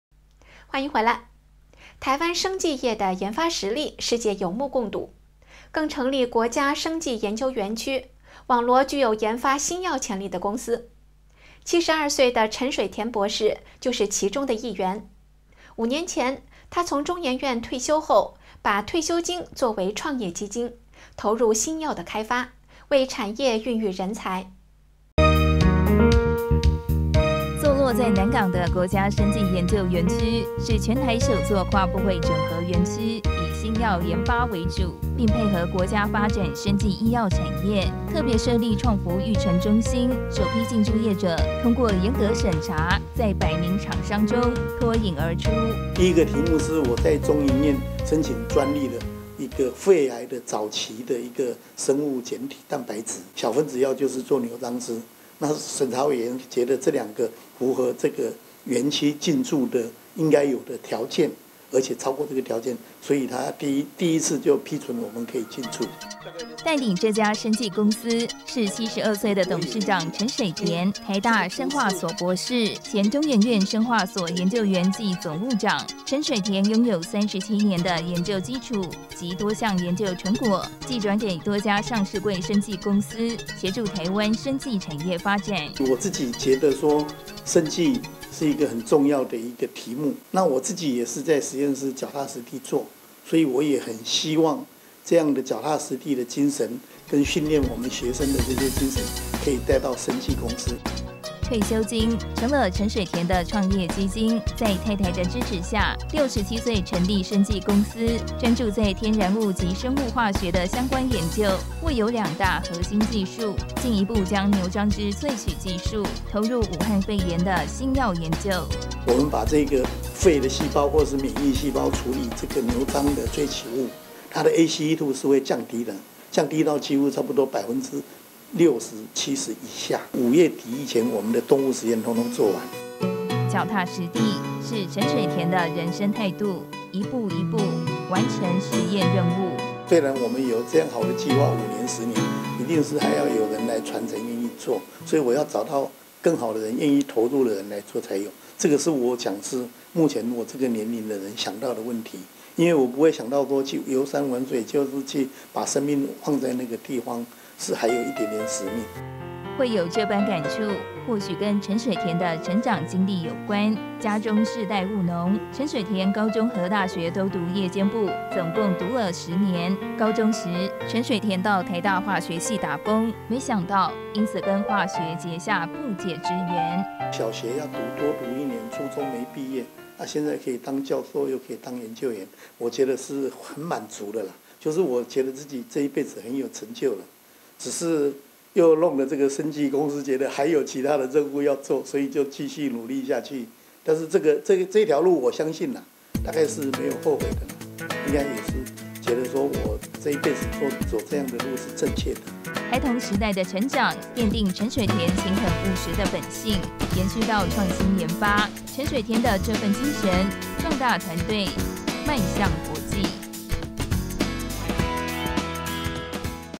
陳水田肩載使命 67歲拿退休金投入新藥開發
台灣生技業研發實力，世界有目共睹，更成立國家生技研究園區，網羅具有研發新藥潛力的公司，72歲的陳水田博士，就是其中的一員。5年前陳水田從中研院退休後，把退休金作為創業基金，投入新藥開發，為產業孕育人才。 坐落在南港的國家生技研究園區，是全台首座跨部會整合園區，以新藥研發為主。並配合國家發展生技醫藥產...